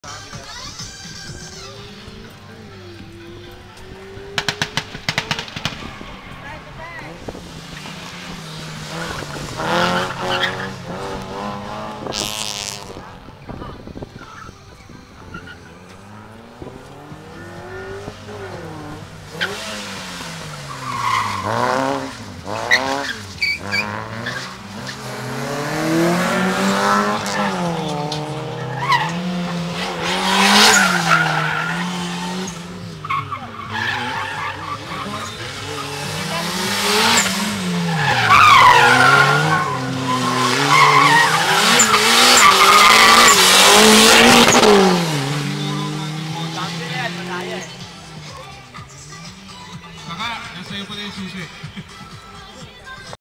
Philippine Autocross Championship Series (2012 Finals)
(!!!Best played at 720p settings, located at the right part of the YouTube player--look for the Cog icon and choose 720p)
The final leg for 2012 season (slalom racing)
Held at the Bonifacio Global City on January 20,2013

Mitsubishi Lancer (Itlog) vs. Honda Civic EK4 Turbo